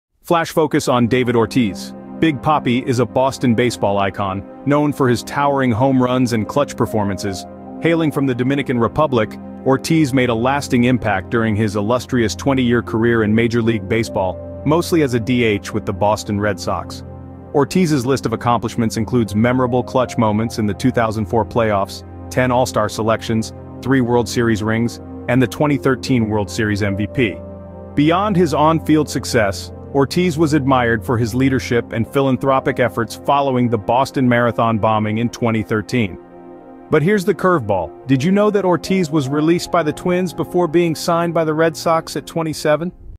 Flash Focus: ⚾️ David Ortiz aka 'Big Papi'
A quick introduction to David Ortiz aka 'Big Papi'. Want to learn more about other ballplayers in our short videos?